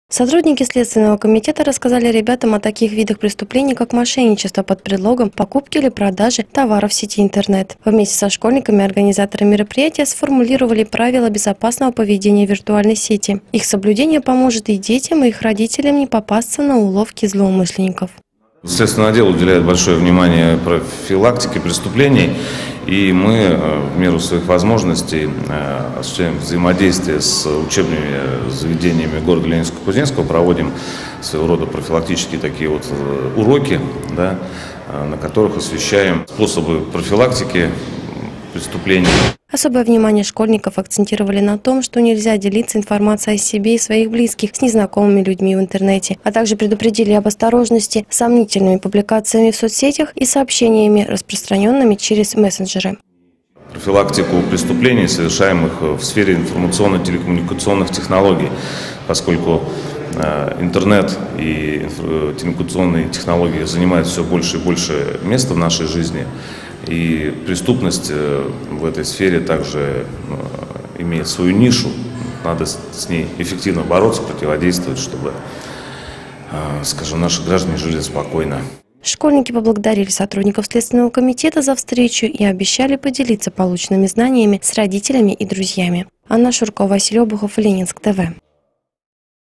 Следственный комитет рассказал школьникам о борьбе с дистанционным мошенничеством.
В целях профилактики распространения мошенничеств, совершаемых с применением дистанционных технологий, сотрудники следственного комитете Ленинска-Кузнецкого провели беседу с учащимися школы №1. Подробности в нашем следующем материале.